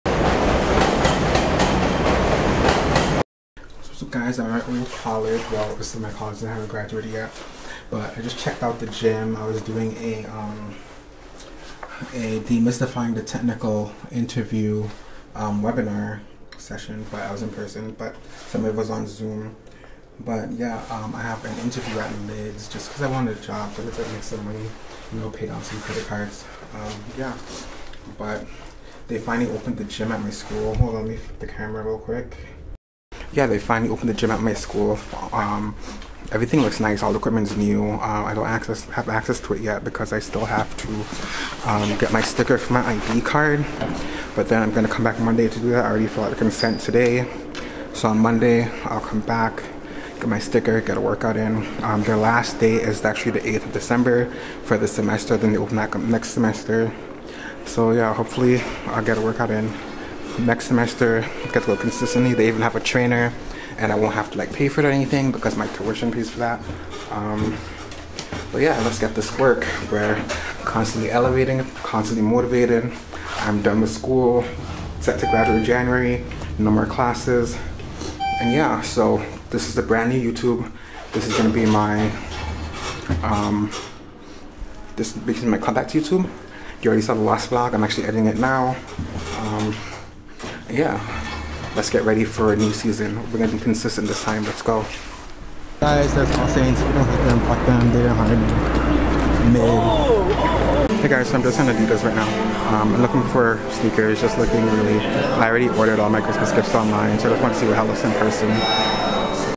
Just a lil Comeback Vlog - College and Interviews
In this vlog, I attend a webinar at my school (CityTech) to learn about interviewing for technical roles. Then I head to Manhattan to interview for a job on 5th avenue.